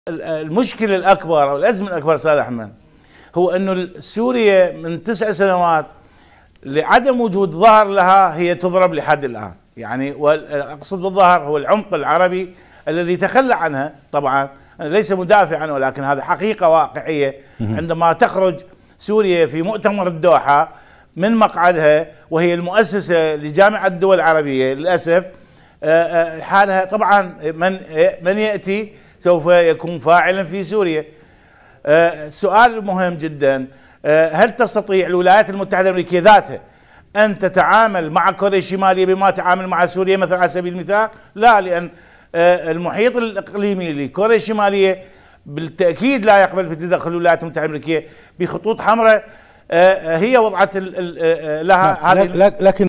نقطة قرار - د .حازم الشمري/ استاذ العلاقات الدولية.لعدم وجود عمق عربي لسوريا مازالت تضرب في الظهر
د .حازم الشمري/  استاذ العلاقات الدولية.
نقطة قرار لتاريخ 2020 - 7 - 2